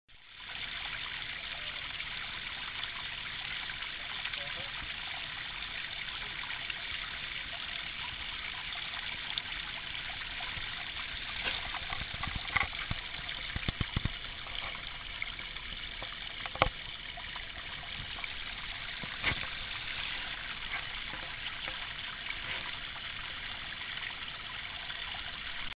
Little stream on Table Mountain
A nice little stream on Table mountain.
16th of october 2011